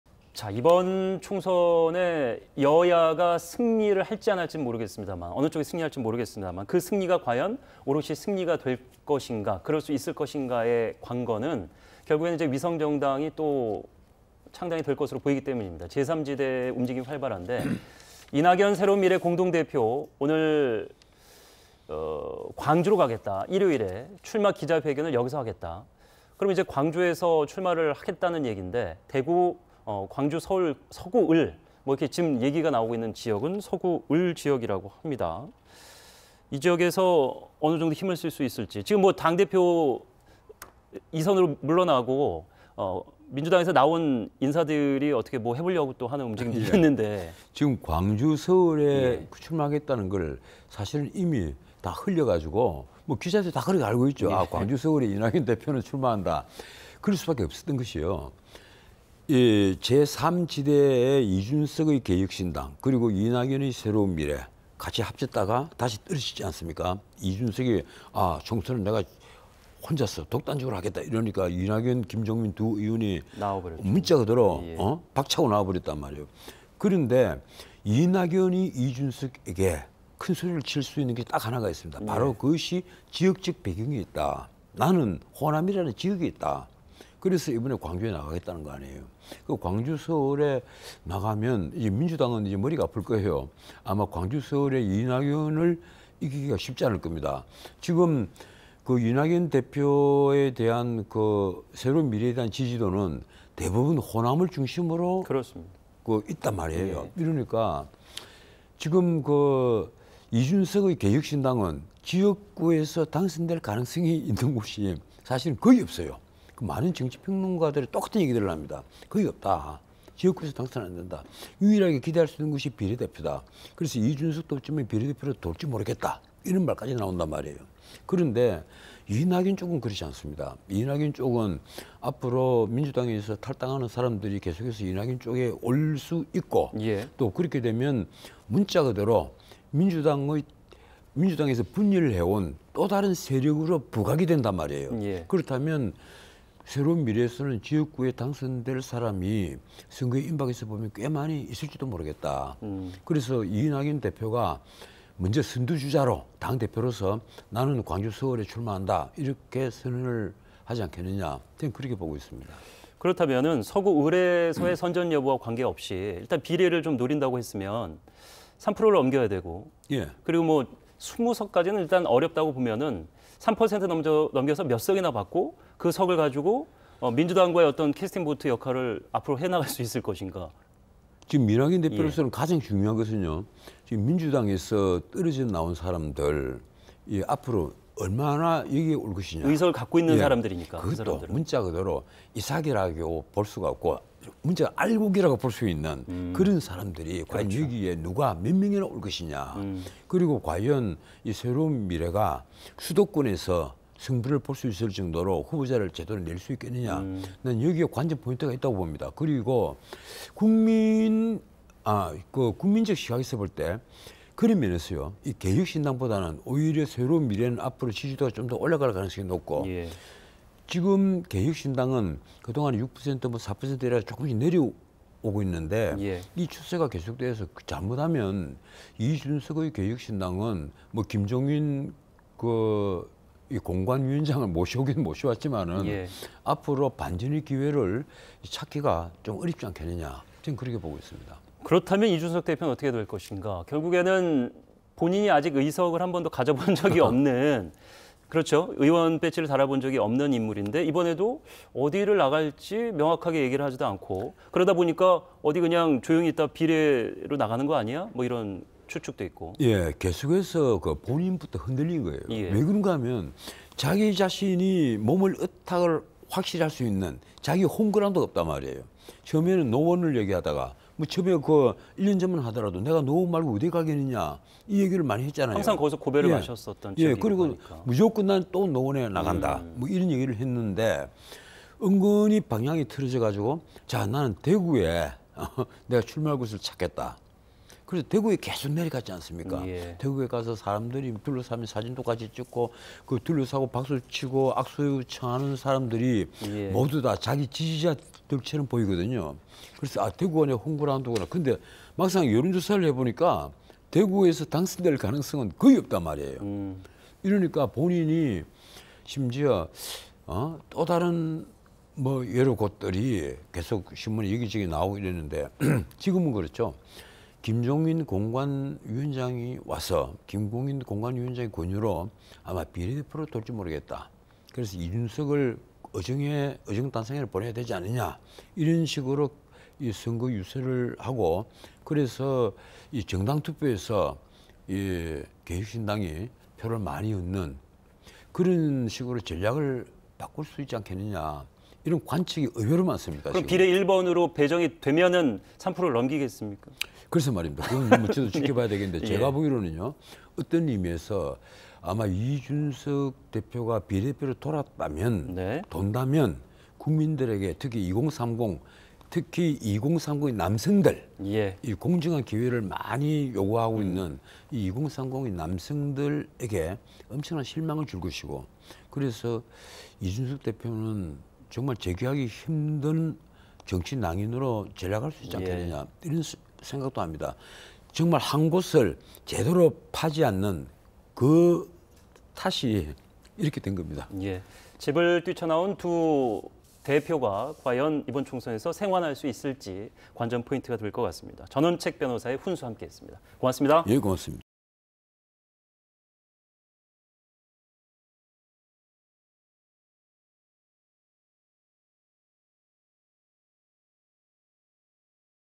의원 배지 없는 이준석... 갈팡질팡하다 결국 비례대표?
이번엔 제3지대 살펴보겠습니다.
이낙연 새로운미래 공동대표가
오는 일요일 광주에서 출마 기자회견을 진행한다고 밝혔습니다.
광주에서 진행하니 출마지는 광주로 보이는데
서구 을이 유력하다는 말이 나오고 있습니다.
개혁신당 양향자 원내대표 지역구로
양 원내대표는 이번에 경기 용인 갑으로 출마하니 무주공산인 상황인데요.
이 공동대표, 광주 서구을에서 선전하겠습니까?